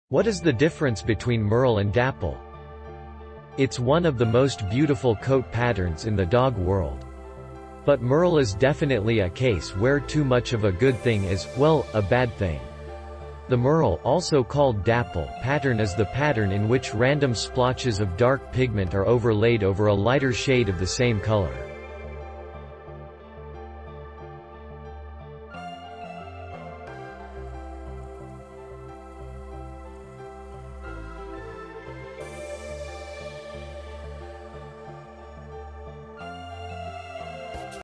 What is the difference between Merle and dapple?
It's one of the most beautiful coat patterns in the dog world. But merle is definitely a case where too much of a good thing is, well, a bad thing. The merle (also called dapple) pattern is the pattern in which random splotches of dark pigment are overlaid over a lighter shade of the same color.

Reference:
The Problem With Merle Coat Patterns in Dogs - Dogster